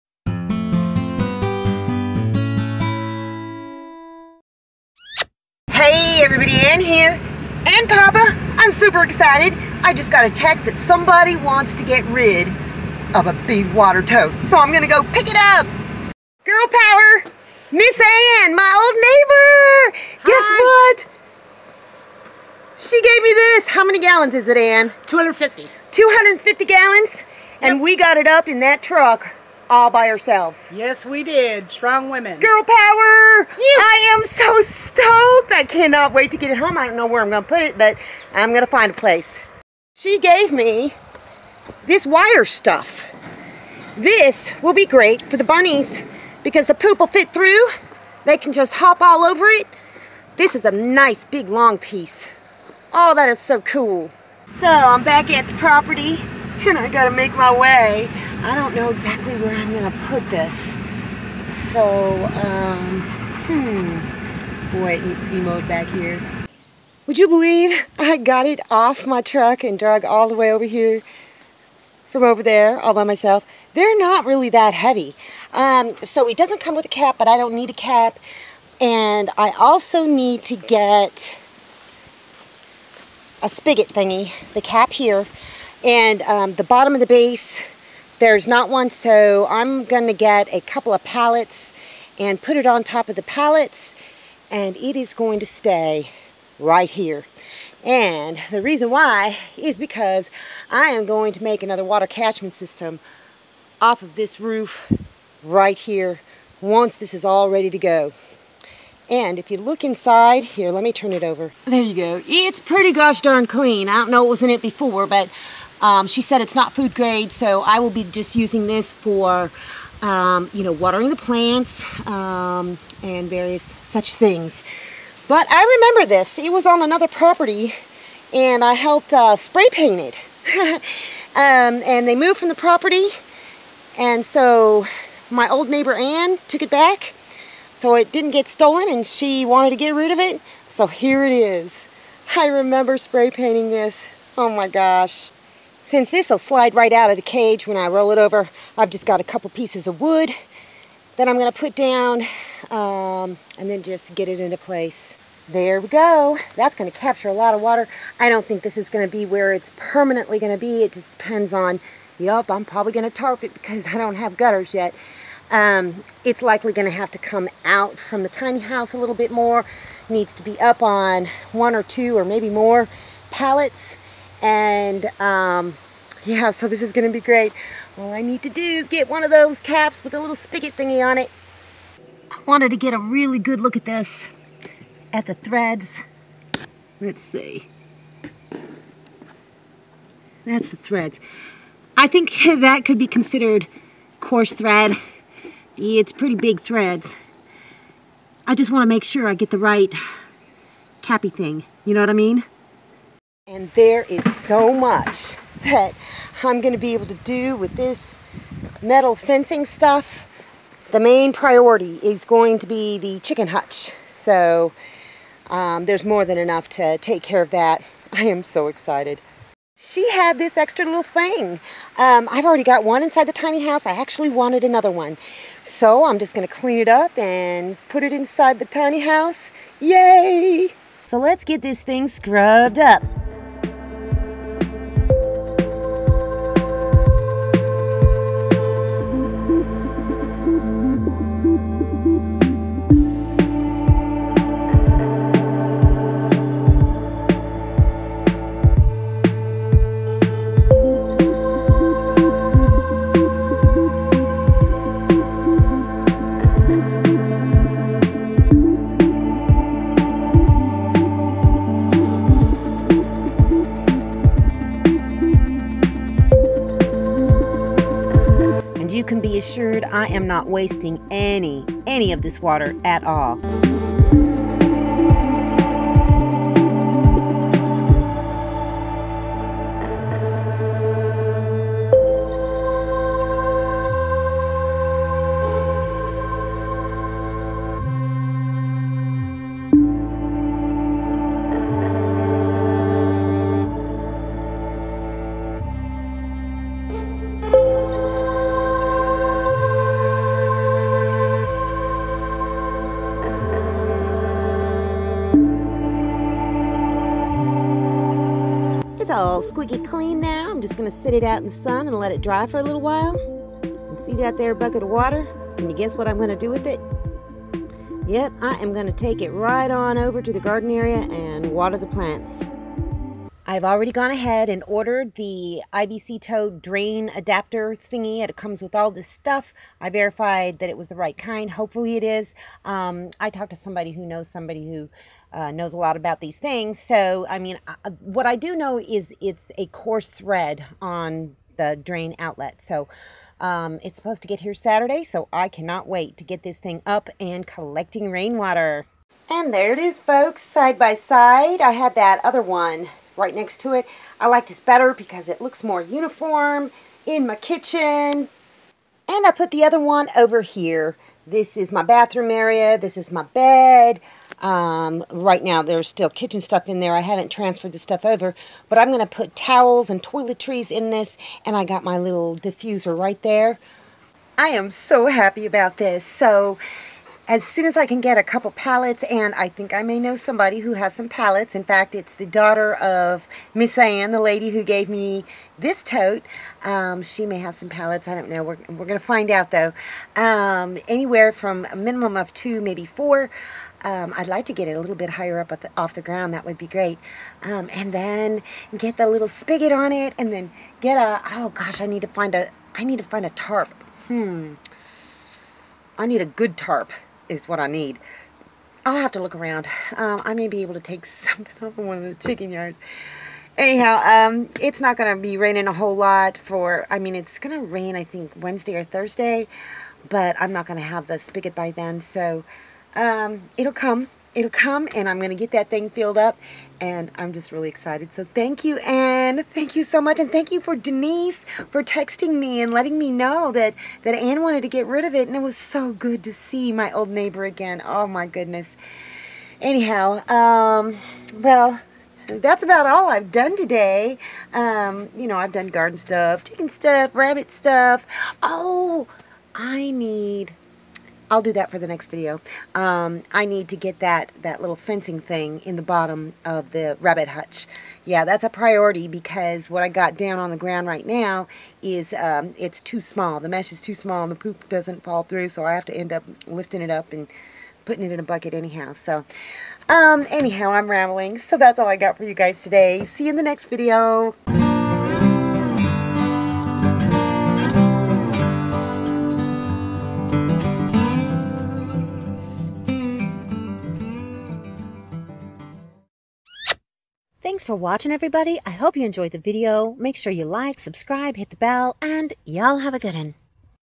Girl Power! - Ann's Tiny Life and Homestead
Got a tip that someone needed to get rid of an IBC tote. Turns out it was my old neighbor! We used girl power to get it up into my truck! She gave me a few other things as well!!!